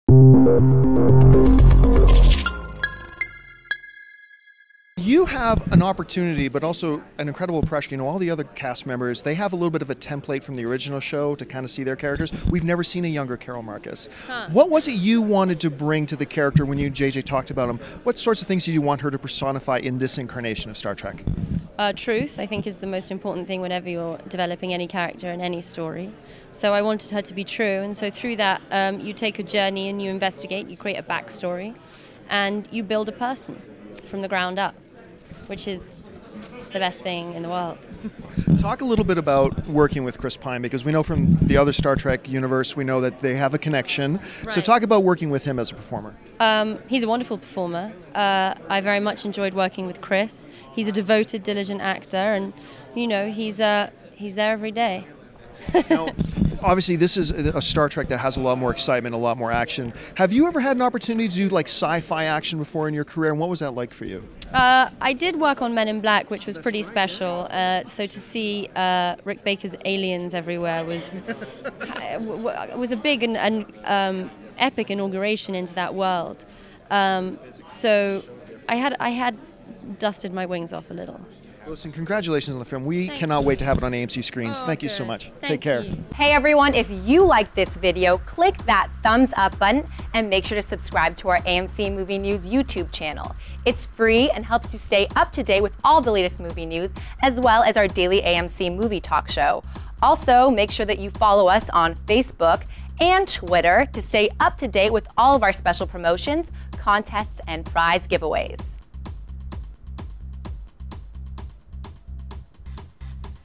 Alice Eve Talks STAR TREK INTO DARKNESS With AMC at CinemaCon 2013
Alice Eve Talks about her upcoming film STAR TREK INTO DARKNESS With AMC at CinemaCon 2013